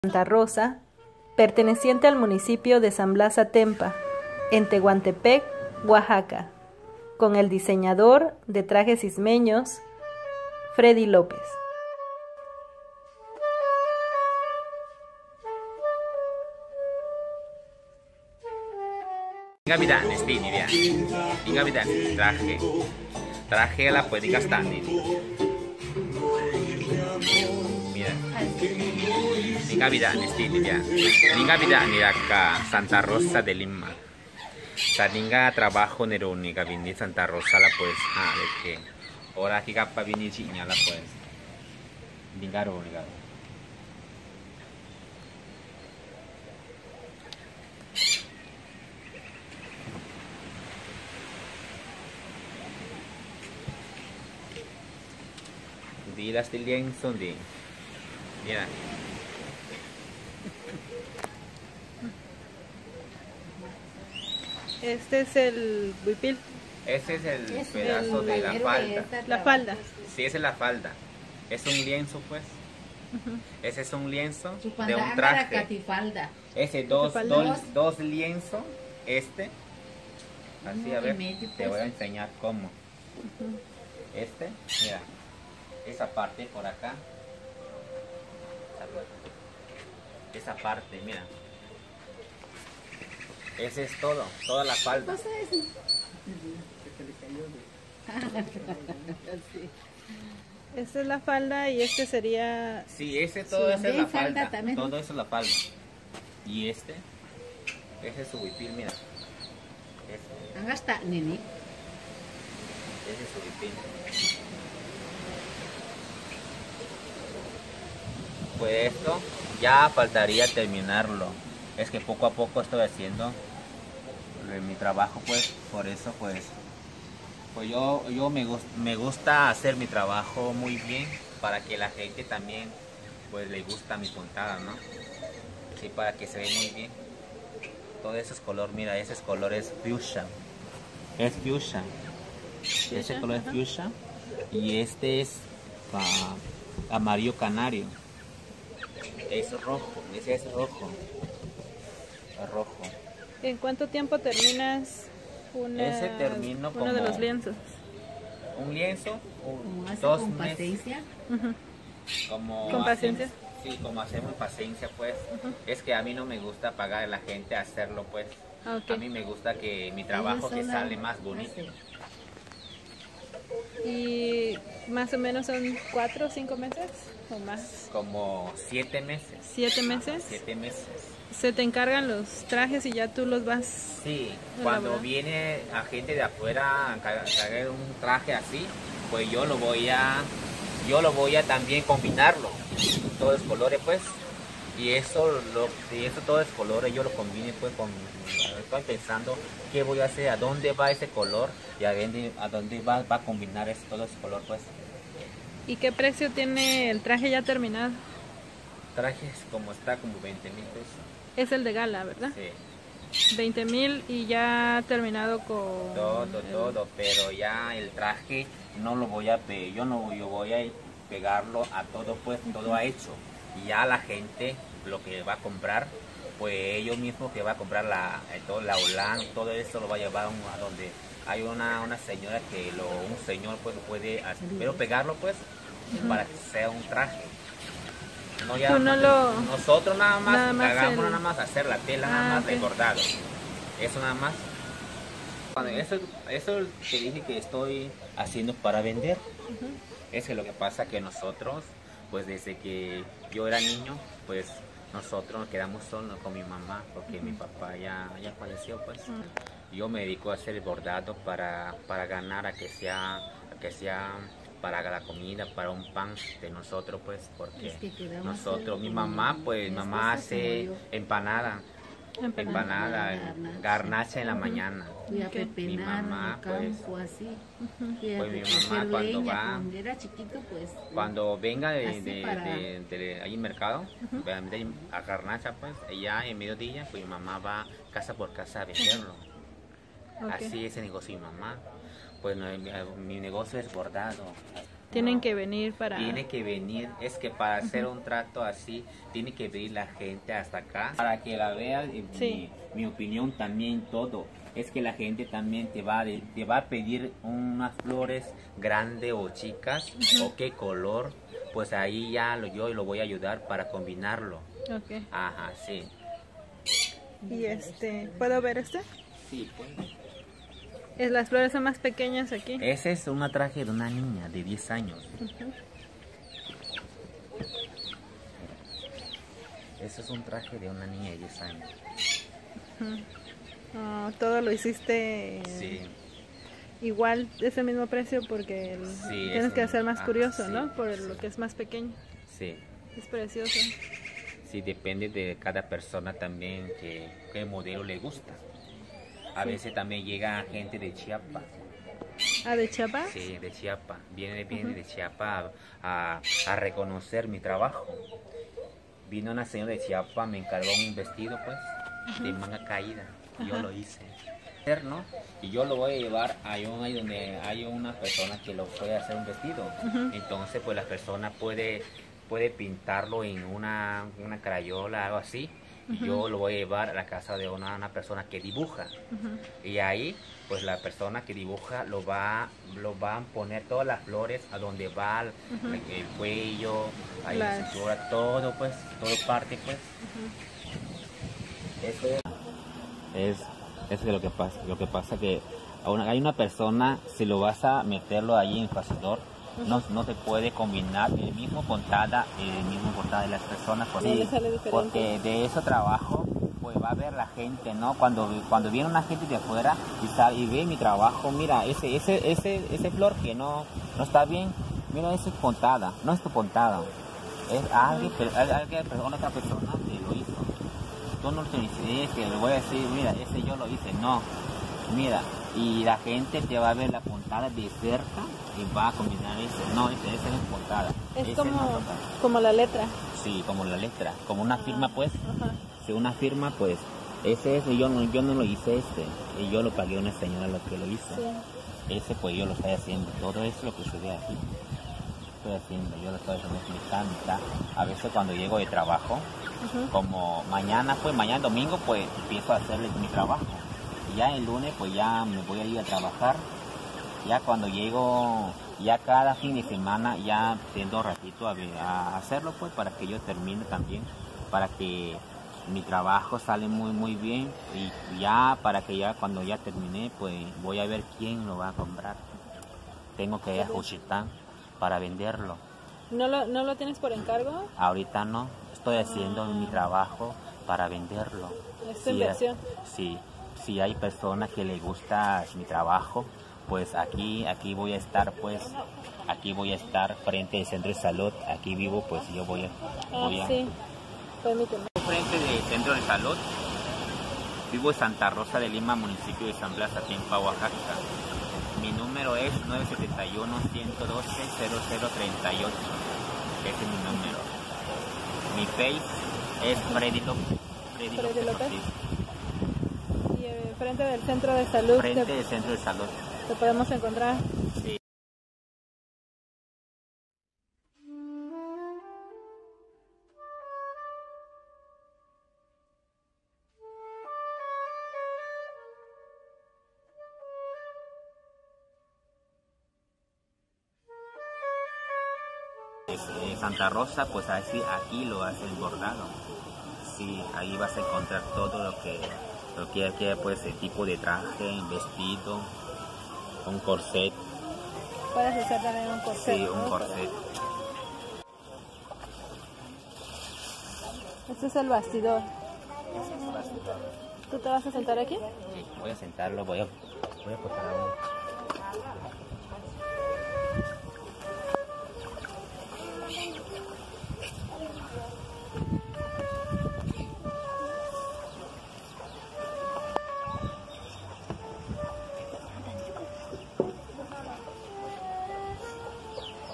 OAXACA!!! Como hacen los Vestidos Bordados En Tehuantepec Oaxaca Mexico!! Diseñador Istmo!
@MexicanaTravel bordados oaxaca
Fredy Lopez Diseñador Oaxaqueno, quien borda los mas preciosos lienzos, para vestidos istmenos y huipiles en Santa Rosa De Lima, San Blas Atempa, Tehuantepec Oaxaca Mexico!
Una tradicion que ha aprendido por herencia. Un trabajo BELLISIMO!